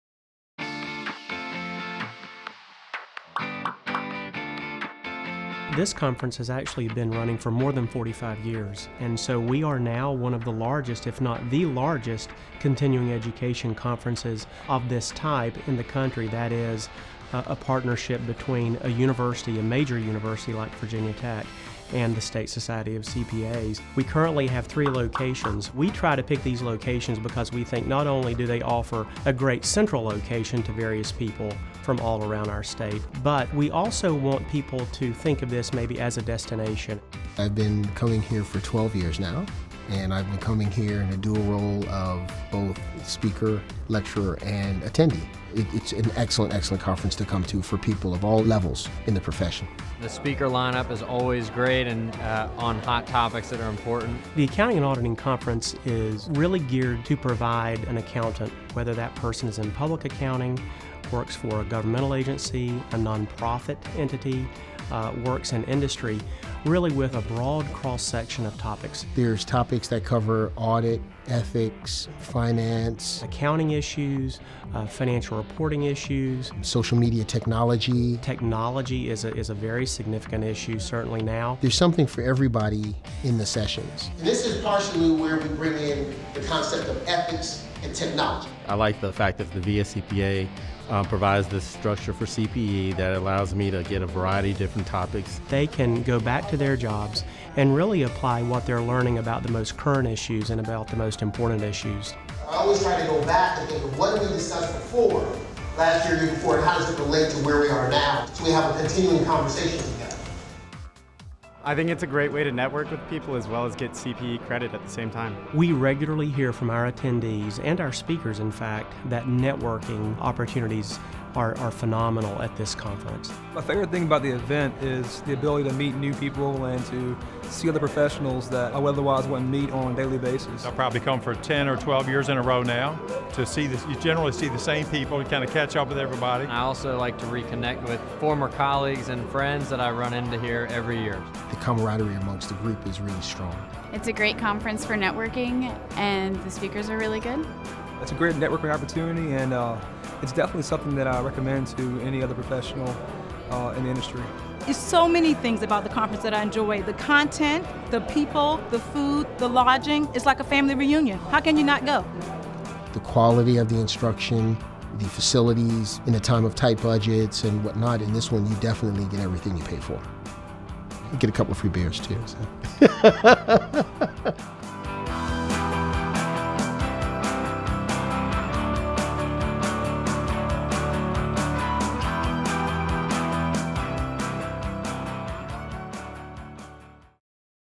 Annual Virginia Accounting & Auditing Conference
For more than 45 years, the Annual Virginia Accounting & Auditing Conference continues to be our most popular event, sponsored by the VSCPA and Virginia Tech, and trusted by more than 850 accounting professionals each year. Receive updates in auditing, tax, fraud, accounting, technology and more.